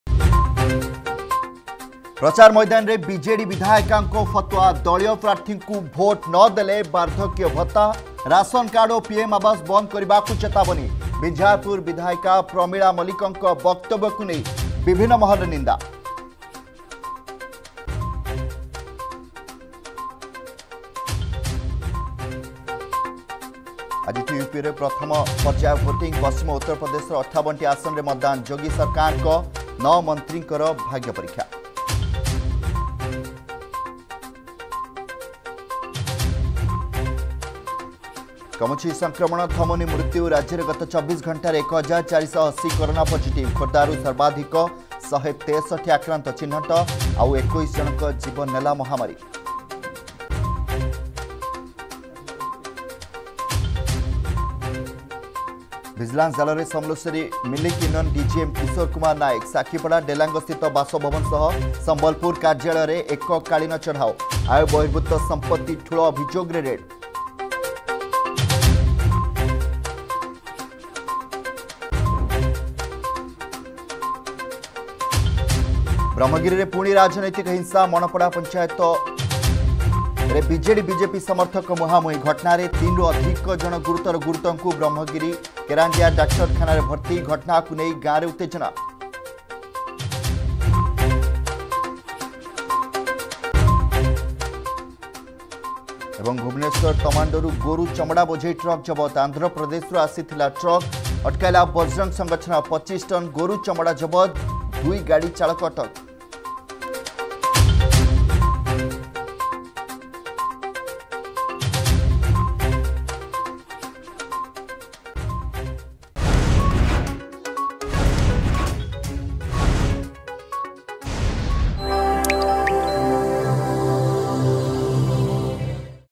Headlines 10 am | 10 Feb 2022 | Argus News
+ପ୍ରଚାର ମୈଦାନରେ ବିଜେଡ଼ି ବିଧାୟିକାଙ୍କ ଫତଓ୍ବା..
+ଆଜି ୟୁପିରେ ପ୍ରଥମ ପର୍ଯ୍ୟାୟ ଭୋଟିଂ...
+କମୁଛି ସଂକ୍ରମଣ, ଥମୁନି ମୃତ୍ୟୁ...
+ଭିଜିଲାନ୍ସ ଜାଲରେ ସମଲେଶ୍ବରୀ ମିଲକ ୟୁନିୟନ DGM କିଶୋର କୁମାର ନାଏକ...
+ବ୍ରହ୍ମଗିରିରେ ପୁଣି ରାଜନୈତିକ ହିଂସା...
+ଭୁବନେଶ୍ବର ତମାଣ୍ଡୋରୁ ଗୋରୁ ଚମଡ଼ା ବୋଝେଇ ଟ୍ରକ ଜବତ...